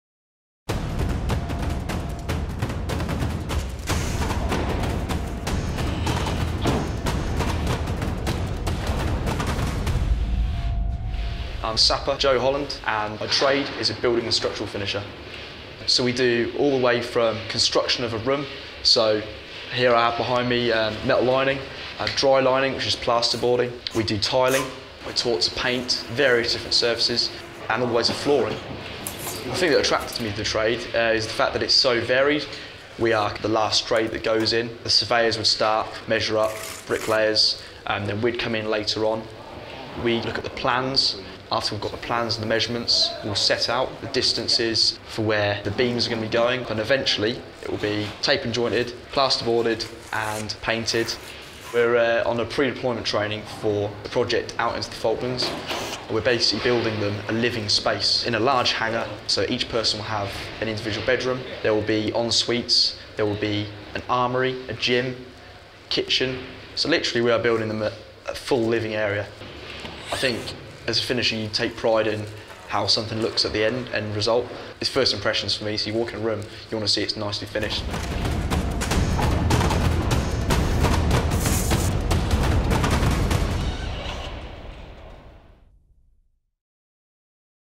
Corps of Royal Engineers - Building & Structural Finisher
This precise and hands-on trade is showcased by B&SF's that serve within the British Army's Corps of Royal Engineers.

Job Role: You will prepare and finish surfaces and be required to complete a newly constructed building or renovate an existing facility. You are responsible for the repair or installation of internal building finishes, from initial preparation to the application of surface and protective finishes including plastering, tiling and painting to walls, floors, ceilings and structural steel.

Qualifications Gained: Level 2 Diploma in Construction Maintenance Operations, Level 2 & 3 Diploma in Building & structural finishing.